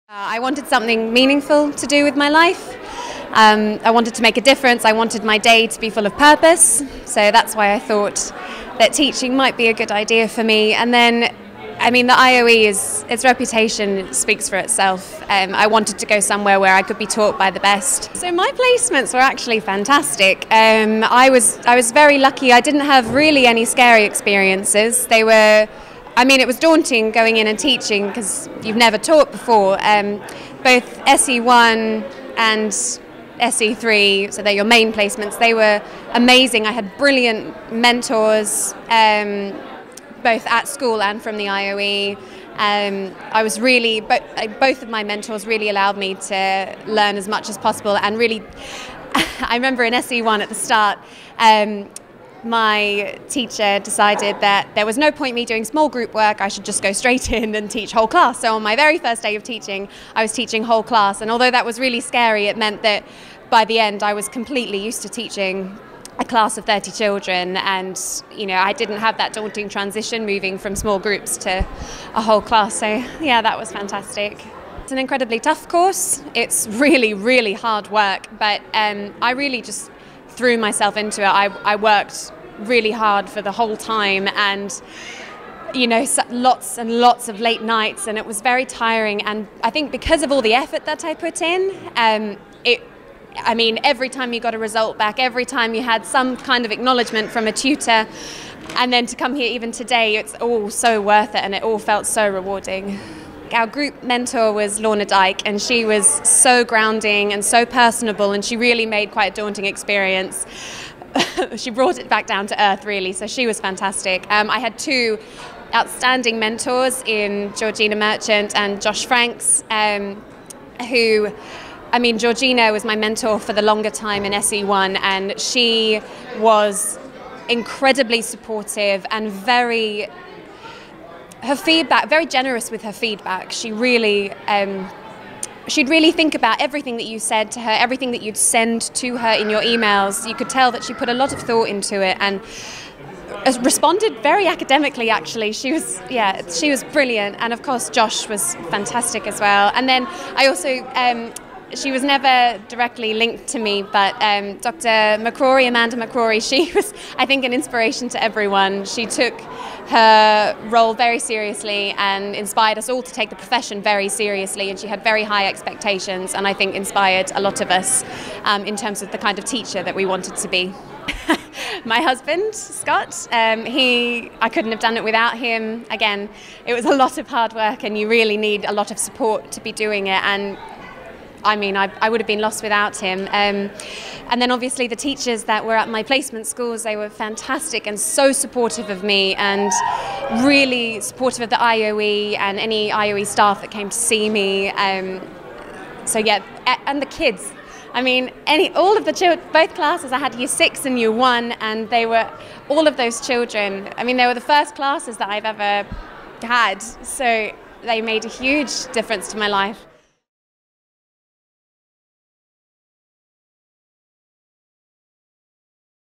Taught by the best | UCL IOE
On graduation day we caught up with Cassandra who recently completed the Primary PGCE course.. She shares stories about her experiences, including what her teaching placements at schools were like, and the support from academics at IOE.

Teacher Training at the IOE, UCL's Faculty of Education and London's Global University